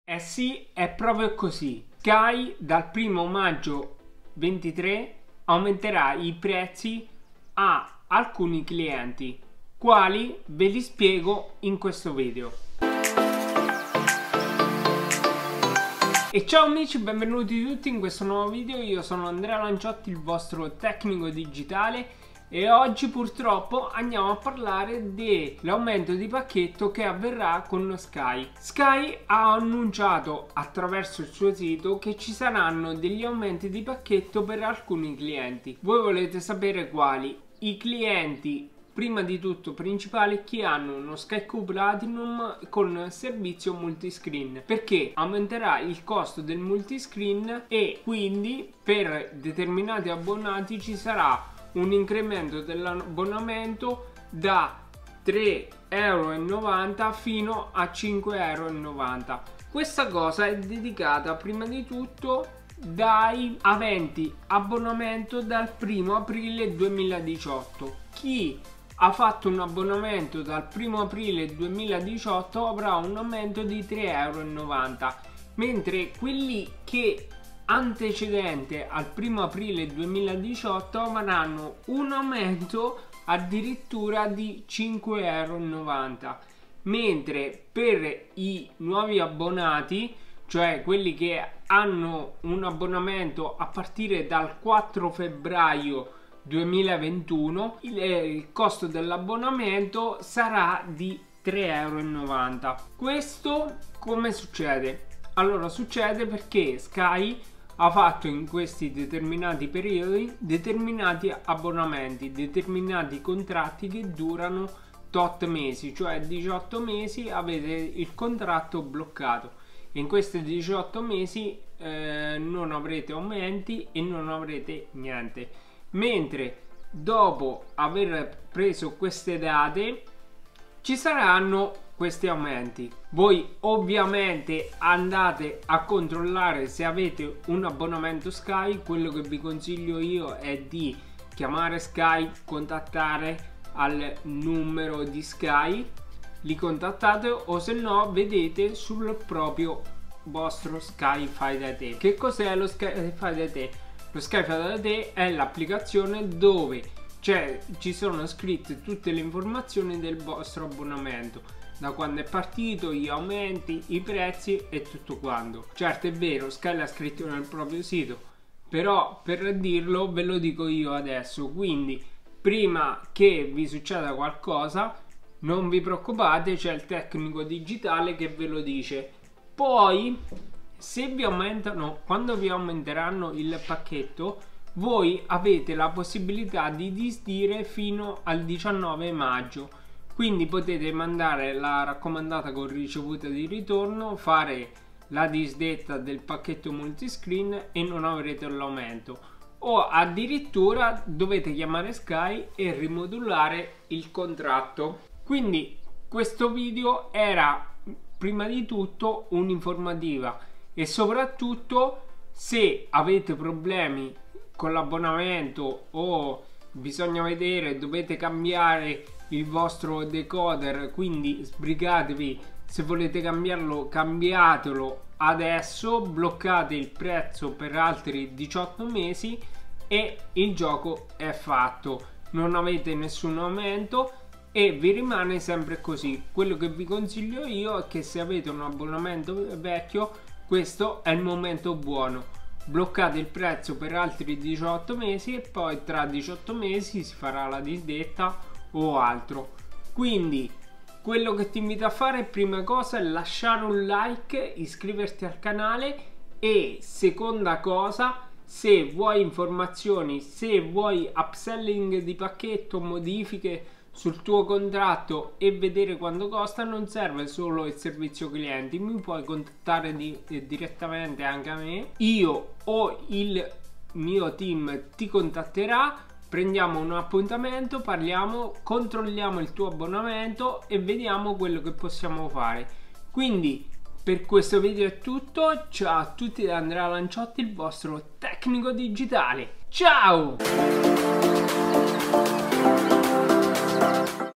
Sky, i nuovi prezzi che fanno discutere analisi e soluzioni
Benvenuti nel mio canale YouTube! Qui troverete tutto ciò che c'è da sapere sugli aumenti dei prezzi di Sky e le soluzioni per risparmiare senza rinunciare ai vostri programmi preferiti.

Analizzeremo insieme i motivi degli aumenti dei prezzi di Sky, le nuove offerte disponibili, le opzioni alternative per coloro che cercano di risparmiare e tanto altro. Inoltre, discuteremo di come ottenere il massimo dal vostro abbonamento Sky, come scegliere la migliore opzione di abbonamento e come negoziare i prezzi con l'azienda.

Il mio obiettivo è quello di fornirvi informazioni utili e pratiche, consigli e trucchi per risparmiare e continuare a godere dei vostri programmi preferiti su Sky.